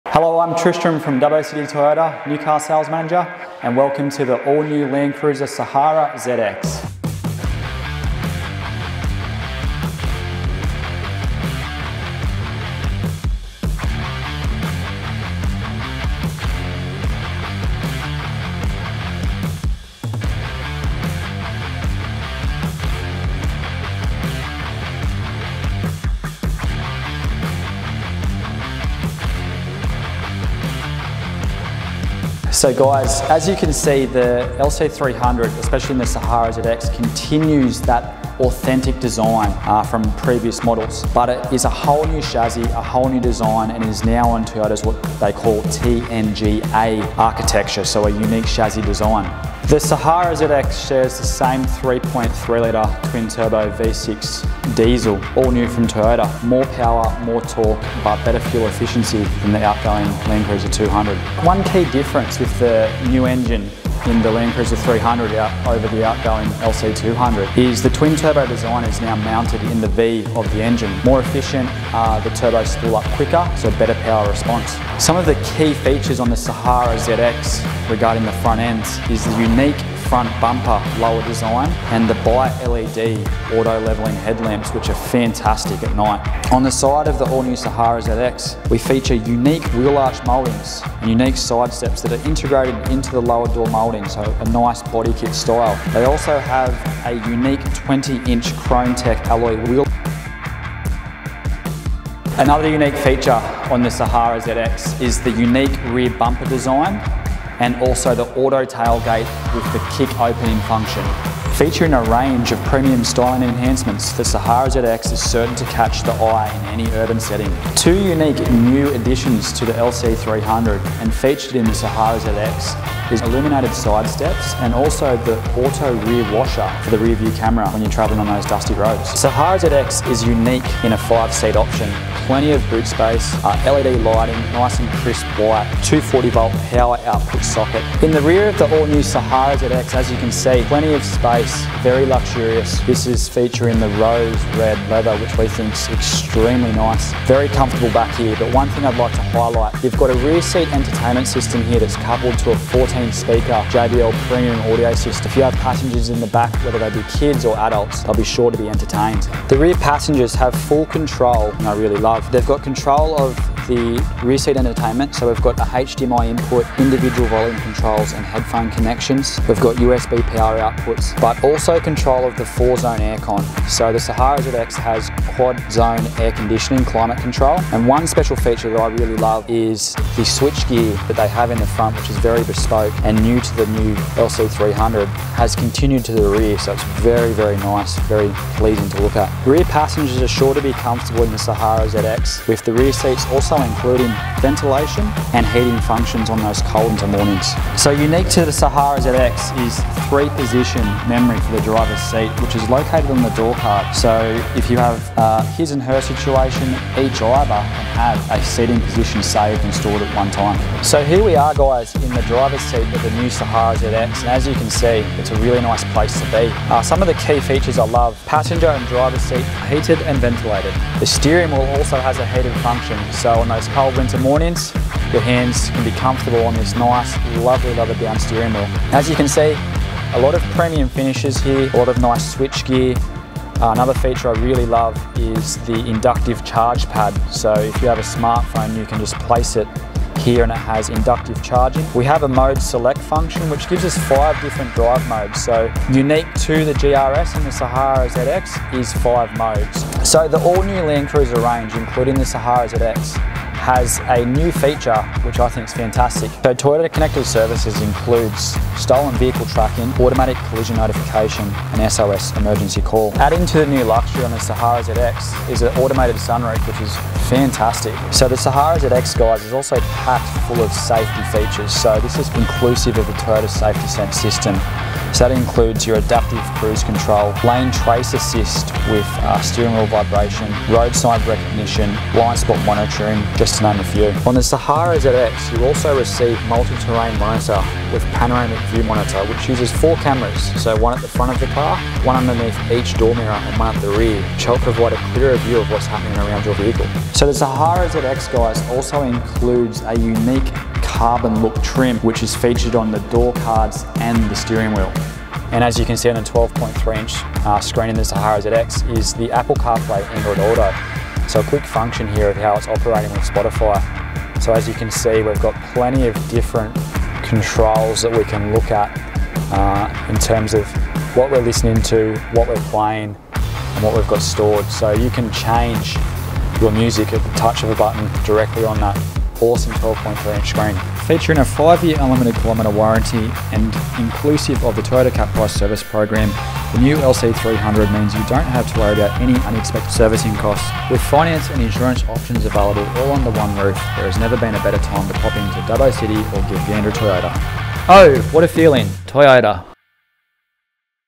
Dubbo City Toyota | 2021 Toyota LandCruiser Sahara ZX | Virtual Vehicle Experience
With the release of the LC300, Tristram goes in depth and explores all the features of the Saraha ZX

Stayed tuned for more Virtual Vehicle Experience!

Skip ahead here!
0:30 - Front Grill
0:48 - Engine Specs and Facts
1:14 - Head Lamps
1:25 - Exterior
1:41 - Rear Tail Gate & Kick Sensor
1:51 - Exterior Styling
2:08 - Boot Space
2:18 - Rear Seats and Technology
3:17 - Front Seats and Technology
4:09 - Toyota Connected Services and Moon Roof
4:27 - Safety Features
4:56 - Interior Design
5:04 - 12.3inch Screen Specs
5:30 - Warranty & Capped Price Servicing